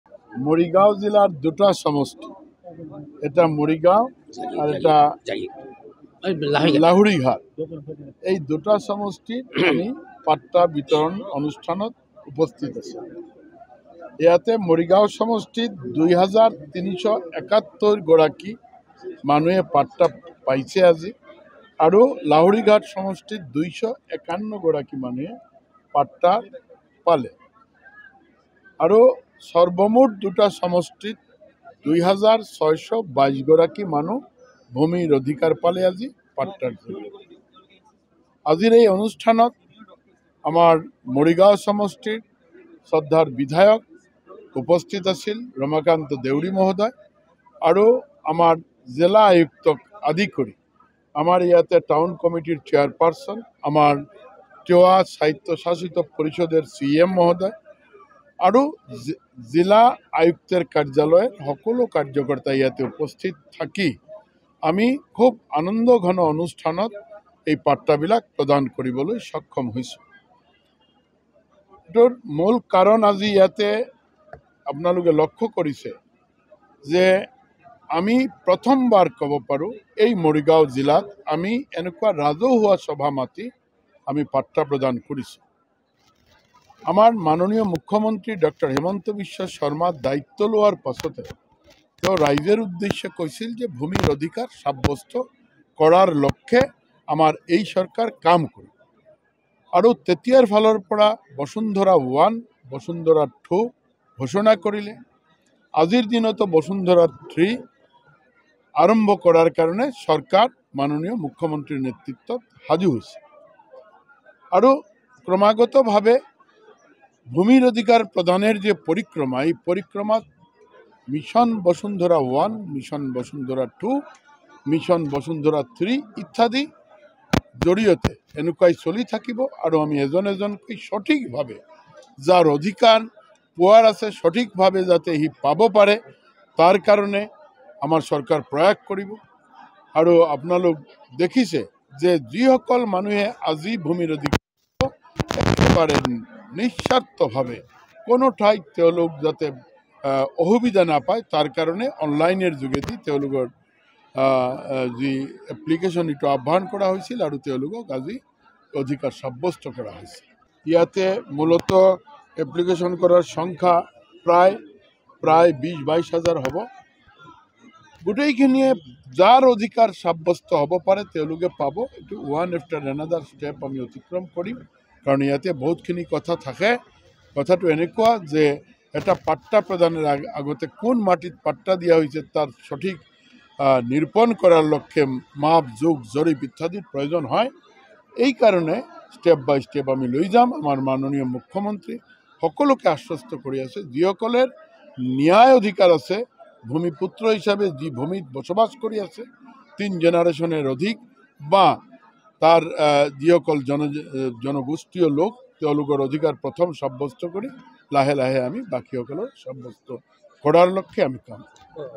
Parimal Suklabaidya News : ৫ হাজাৰৰো অধিক লোকক ভূমি পট্টা প্ৰদান অনুষ্ঠান | N18V
Parimal Suklabaidya News | ভূমি অধিকাৰ প্ৰদানেৰে খিলঞ্জীয়া জনগোষ্ঠীৰ সৱলীকৰণ । দেওবাৰে মৰিগাঁও জিলাৰ ৫ হাজাৰৰো অধিক লোকক মিছন বসুন্ধৰা ২.০ ৰ অধীনত ভূমি পট্টা প্ৰদান অনুষ্ঠান । মৰিগাঁও আৰু লাহৰীঘাট সমষ্টিৰ বাবে আজি মৰিগাঁৱৰ তৰুণ ৰাম ফুকন খেলপথাৰত দিনৰ ১২ বজাত আবকাৰী, মীন আৰু পৰিবহণ বিভাগৰ মাননীয় মন্ত্ৰী পৰিমল শুক্লবৈদ্য ডাঙৰীয়াই মাটিৰ পট্টা প্ৰদান কৰে । লগতে উপস্থিত থাকে মৰিগাঁও সমষ্টিৰ বিধায়ক ৰামাকান্ত দেউৰী , জিলা আয়ুক্ত দেৱাশীষ শৰ্মা , তিৱা স্বায়ত্ব পৰিষদৰ CEM জীৱন চন্দ্ৰ কোঁৱৰ , মৰিগাঁও পৌৰসভাৰ পৌৰপতি যোগেশ তামুলী ।